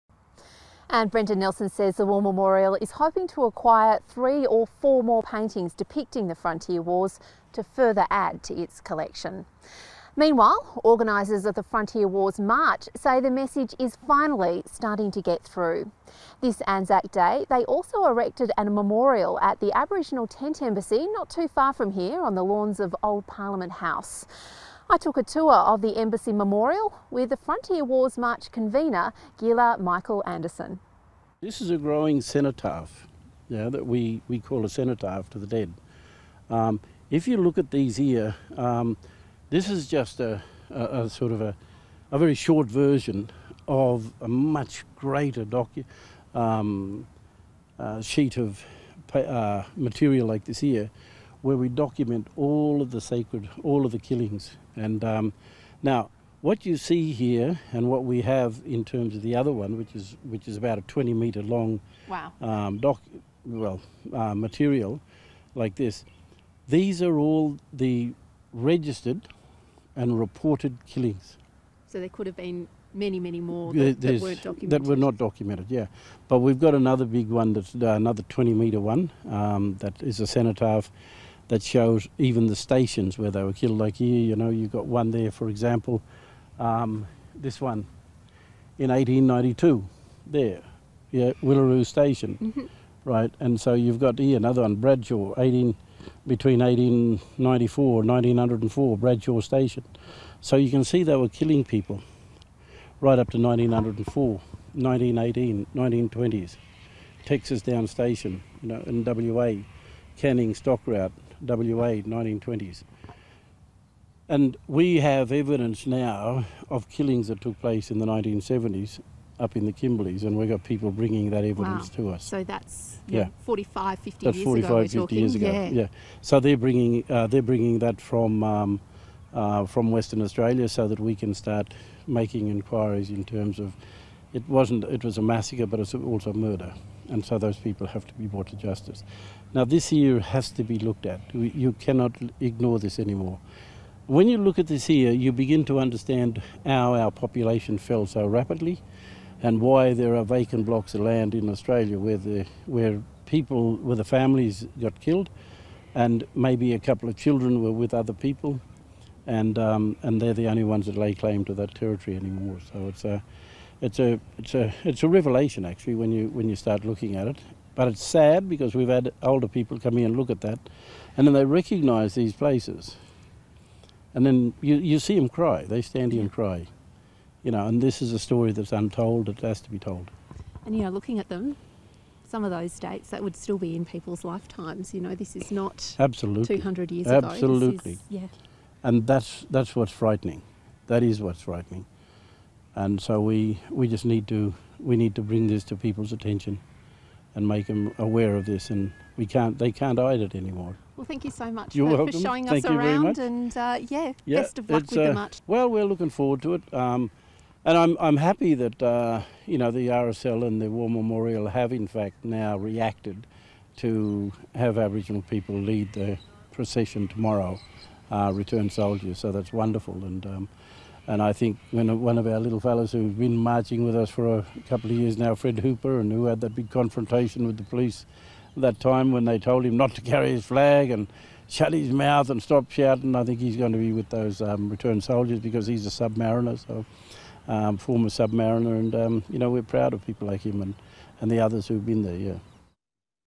Aboriginal Tent Embassy Memorial honours Frontier Wars warriors
Organisers of the Frontier Wars march say that the message is finally starting to get through. This ANZAC Day they also erected a memorial at the Aboriginal Tent Embassy, on the lawns of Old Parliament House. NITV 's Natalie Ahmat took a tour of the Embassy Memorial with the Frontier Wars March convenor, Ghillar Michael Anderson.